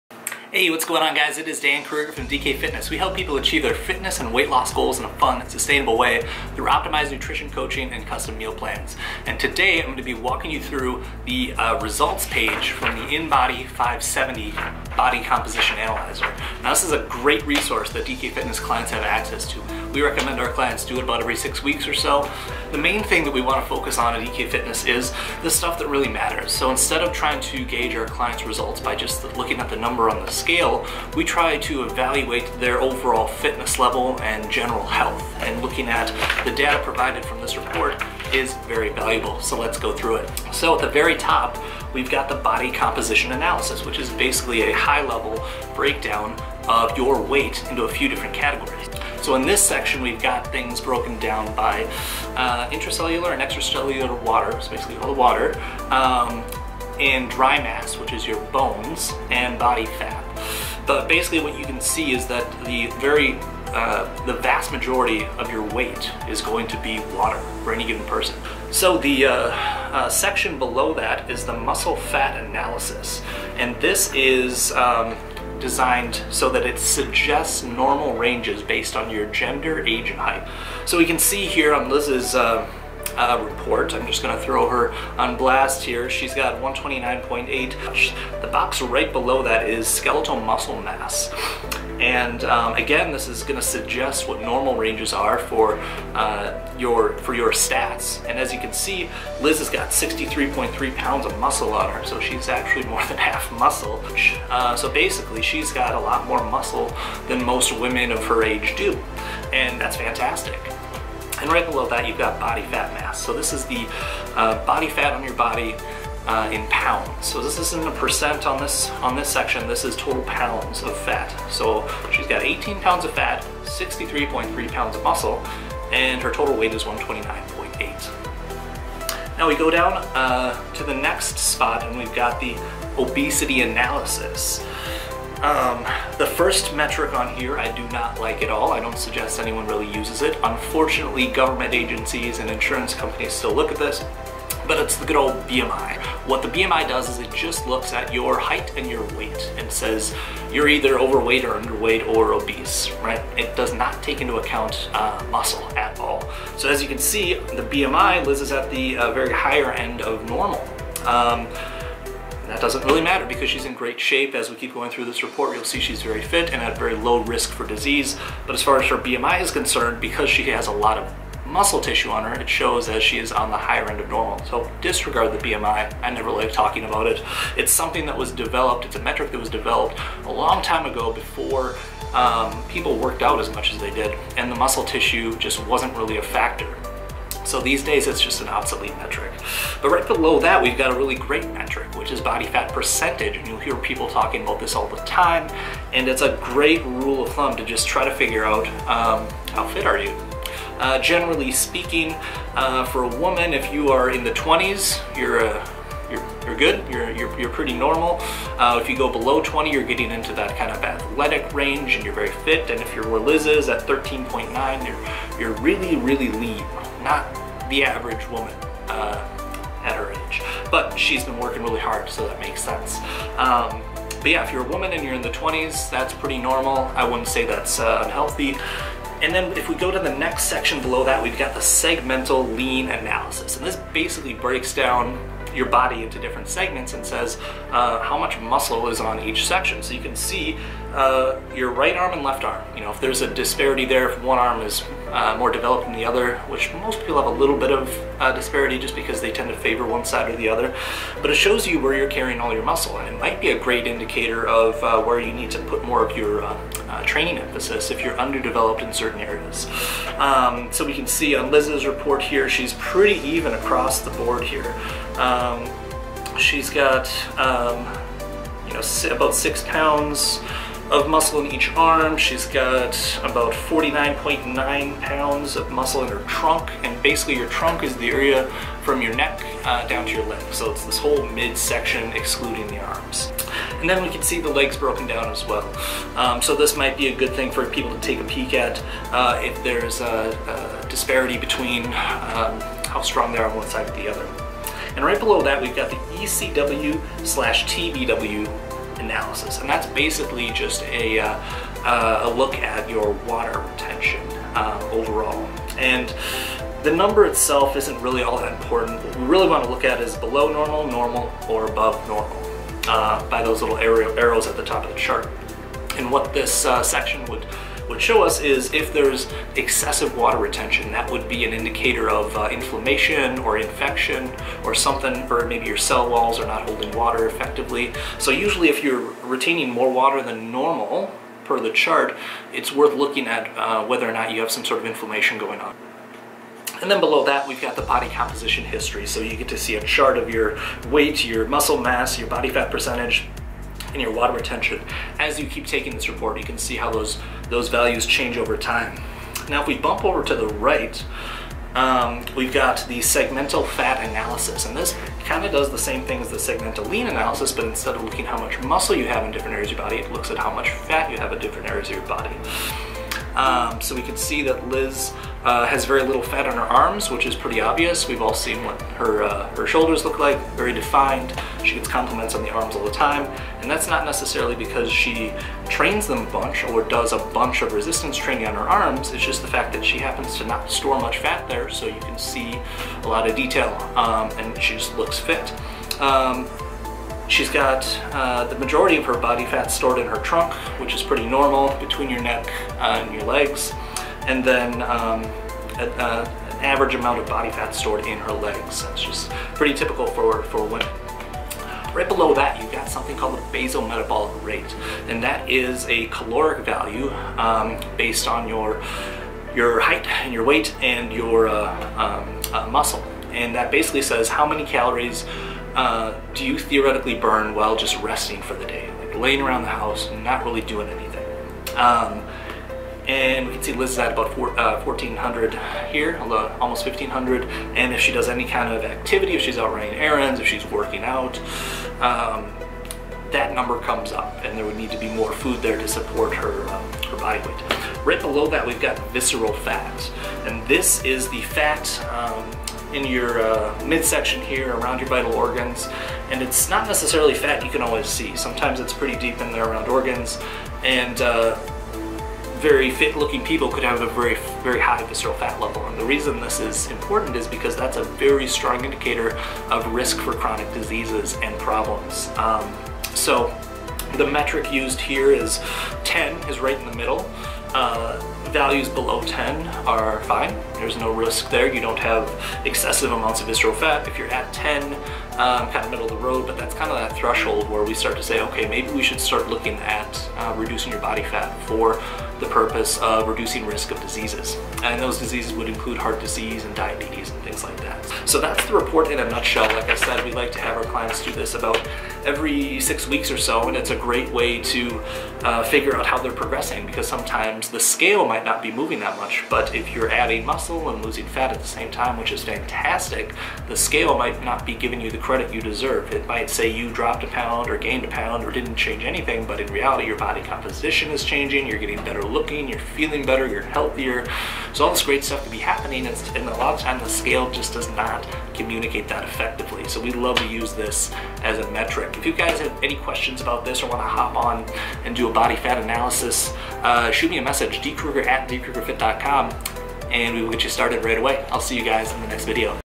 InBody 570 Results Explained
DK Fitness Clients get access to the InBody 570 Body Composition Analyzer. This device produces a report that breaks down your body composition in great detail. In this video, Dan walks you through Liz's recent scan and explains all the elements of the report.

Want to take your results to the next level?!